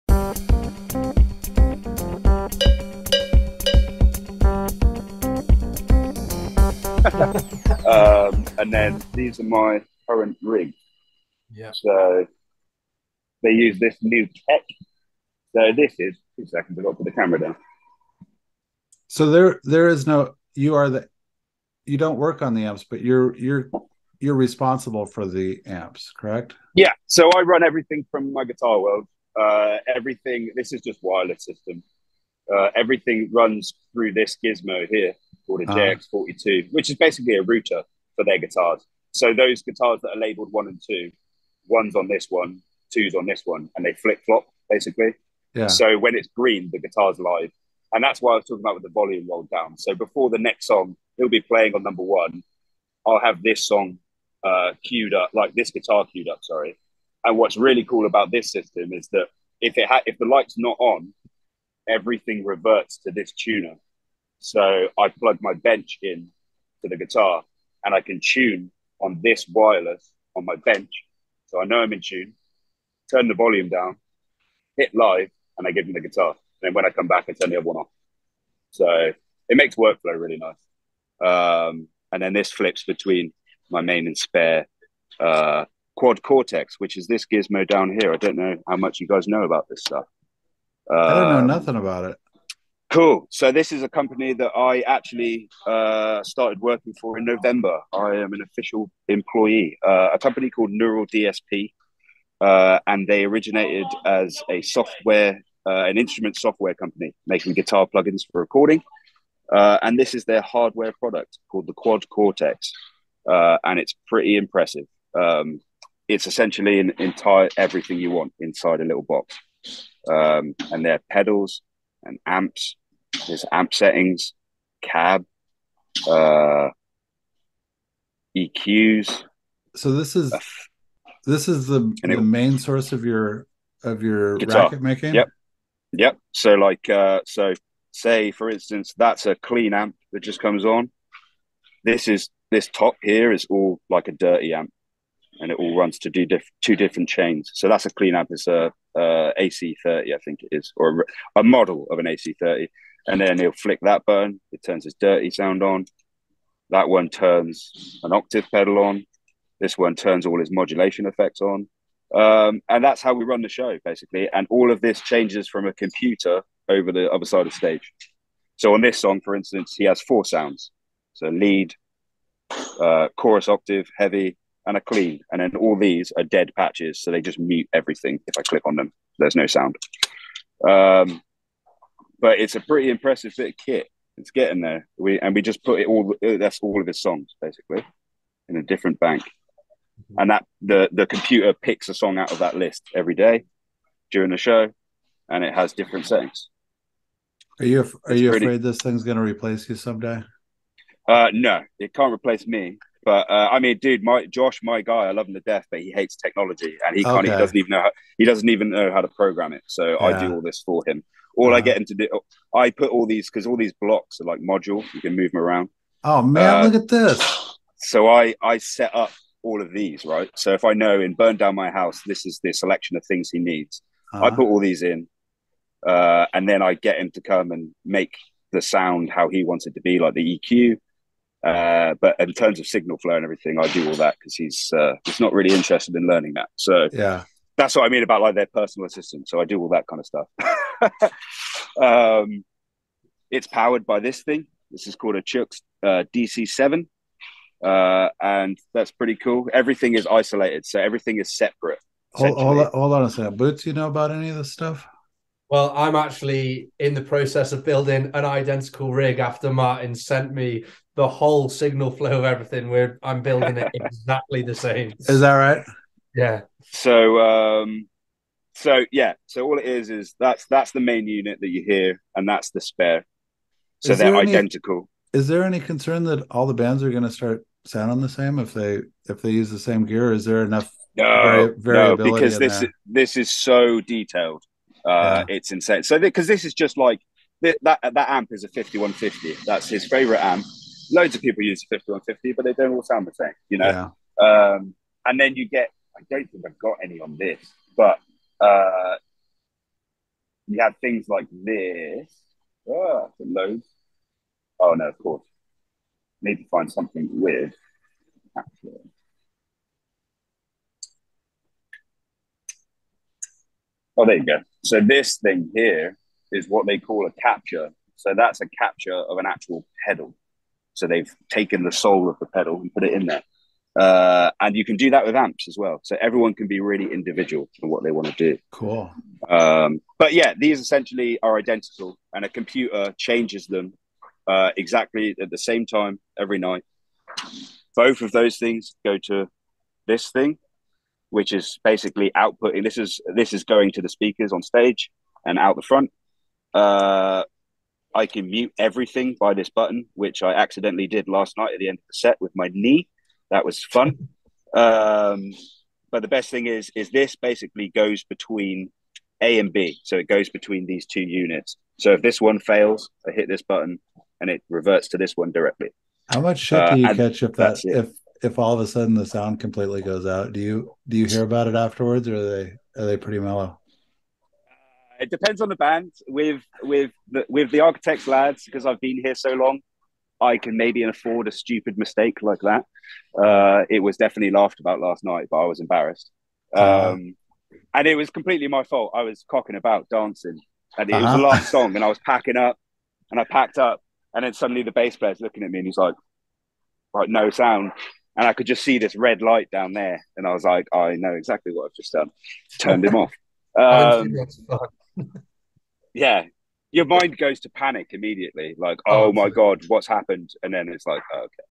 Boots Interviews Martyn Evans Neural DSP
Martyn Evans, out with Architects, show off the functionality of the Neural DSP rig.